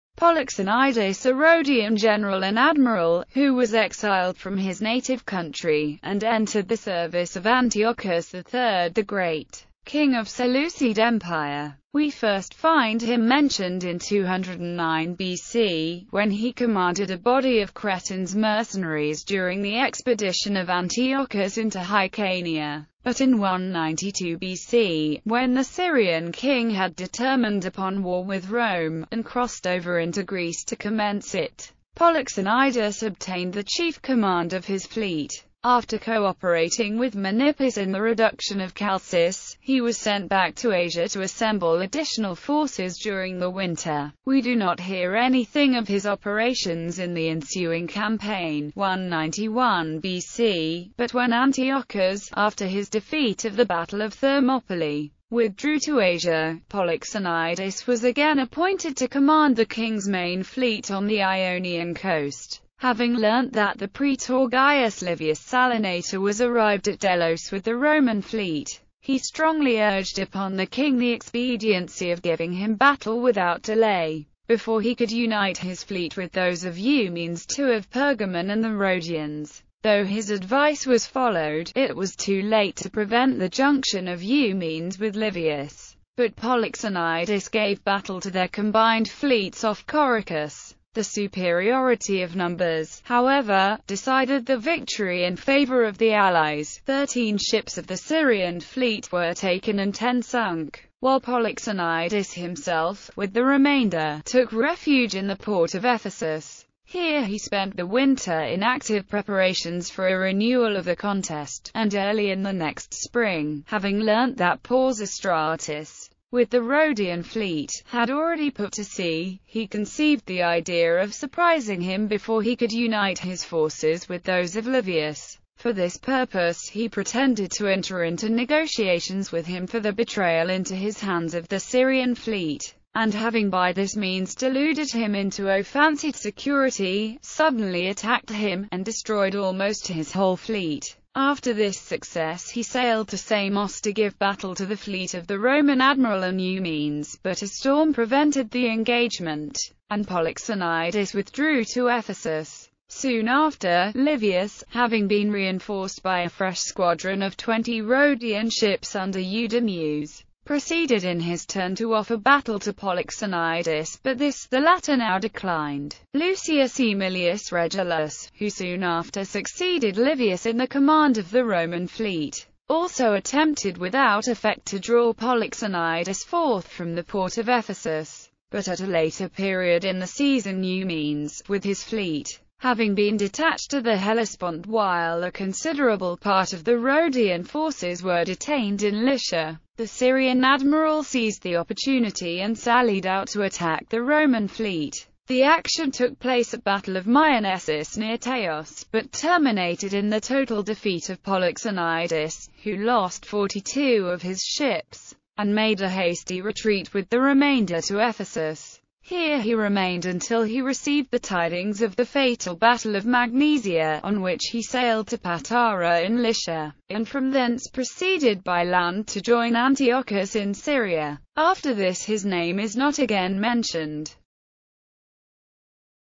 Polyxenidas
Polyxenidas a Rhodian general and admiral, who was exiled from his native country, and entered the service of Antiochus III the Great, king of Seleucid Empire.